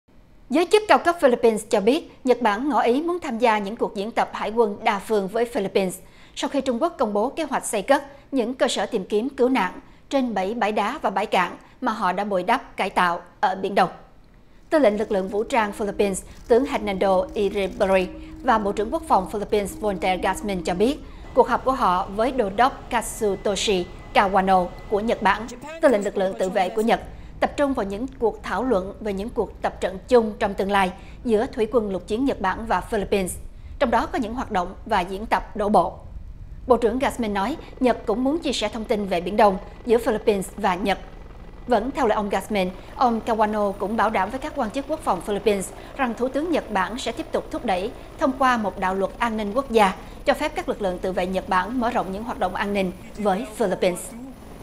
Nhật Bản muốn diễn tập hải quân đa phương với Philippines
Tin tức Nếu không vào được VOA, xin các bạn hãy vào để vượt tường lửa. Các quan chức cao cấp và chuyên gia quân sự của Philippines cho biết Nhật Bản ngỏ ý muốn tham gia những cuộc diễn tập hải quân đa phương với Mỹ và Philippines, sau khi Trung Quốc công bố kế hoạch xây cất những cơ sở tìm kiếm và cứu nạn trên bảy bãi đá và bãi cạn mà họ đã bồi đắp cải tạo ở Biển Đông. Tư lệnh Lực lượng Vũ trang Philippines, Tướng Hernando Iriberri, và Bộ trưởng Quốc phòng Philippines Voltaire Gazmin cho biết cuộc họp của họ với Đô đốc Katsutoshi Kawano của Nhật Bản, Tư lệnh Lực lượng Tự vệ của Nhật Bản, tập trung vào những cuộc thảo luận về những cuộc tập trận chung trong tương lai giữa Thủy quân lục chiến Nhật Bản và Philippines, bao gồm những hoạt động và diễn tập đổ bộ.